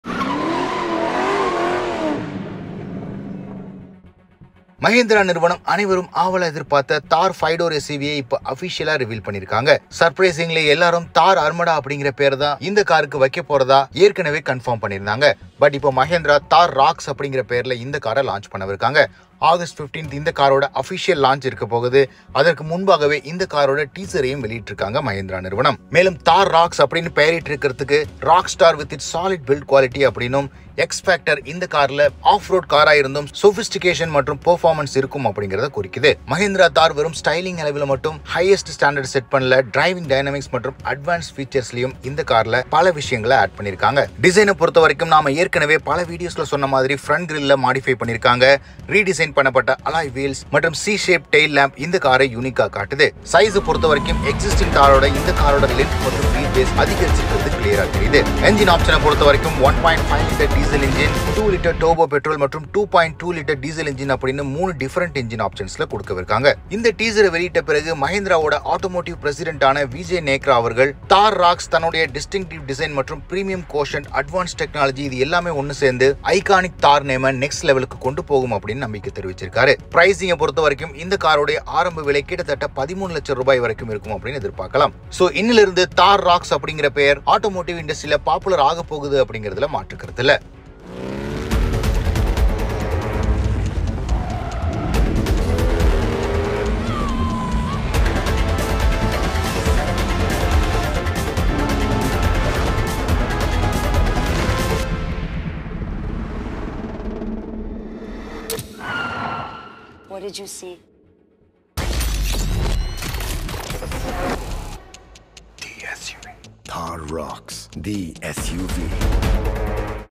Live update💥ஆச்சரிய படுத்திய மஹிந்திரா💥Mahindra Thar Roxx 5 door version
The biggest news for Mahindra fans has to be the launch date of the Mahindra Roxx. So with the official teaser, Mahindra has confirmed that the Roxx SUV will be arriving on 15th of August 2024! Now will the brand launch the Roxx on Independence Day or will it just unveil the SUV? Well, stay updated with MotorOctane as we will be bringing all the new information. Now, let’s talk about the design that has been revealed by Mahindra.